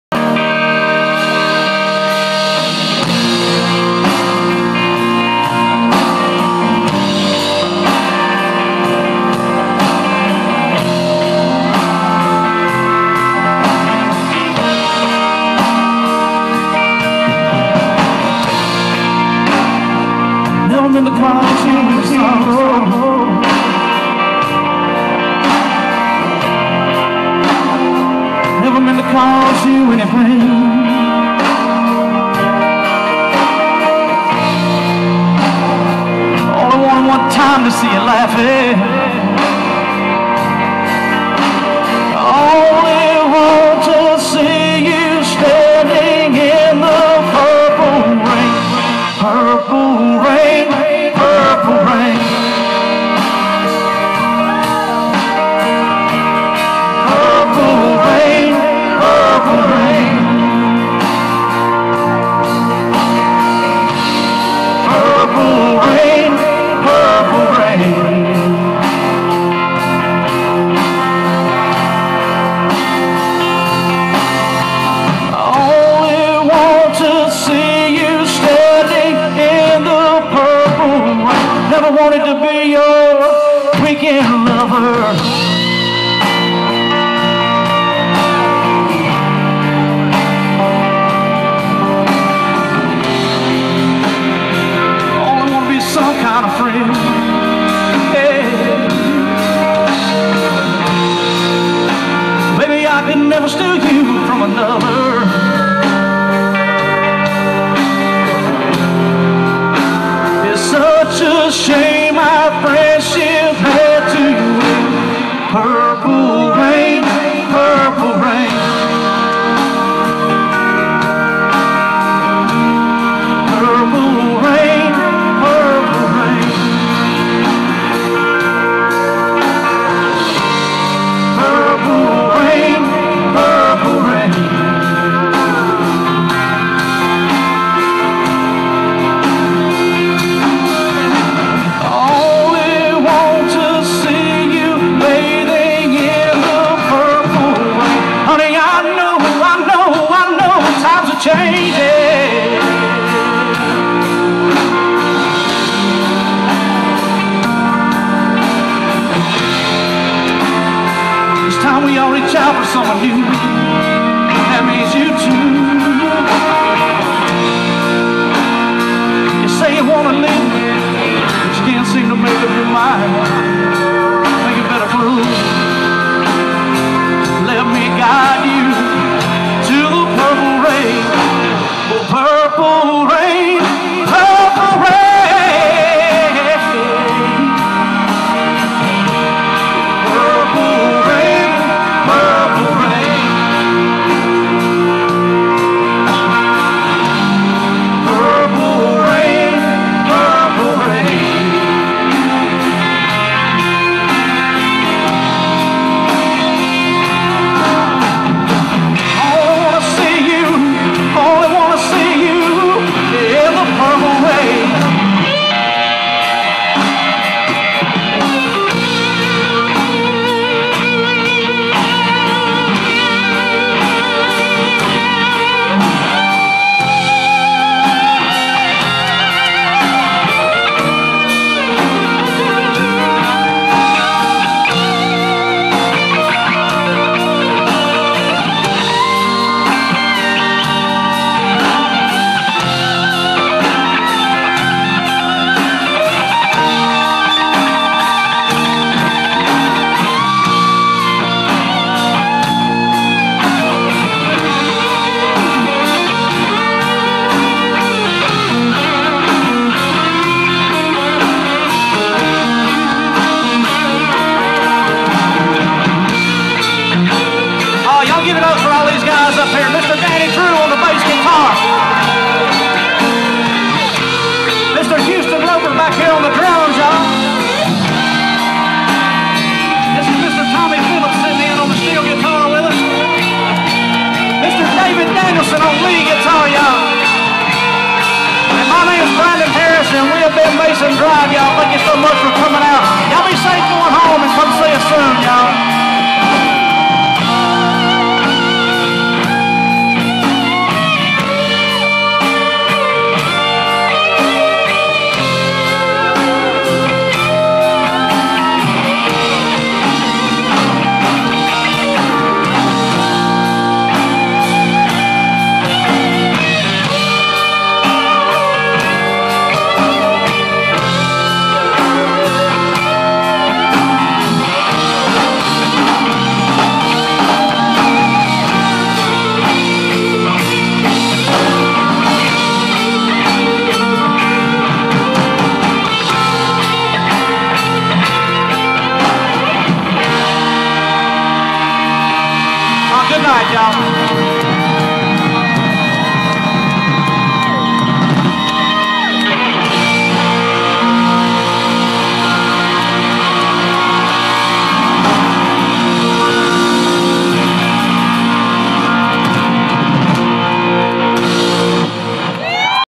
Brandon Harris at Music on Main
Brandon Harris with Mason Drive singing a Prince classic at Music on Main, May 2011.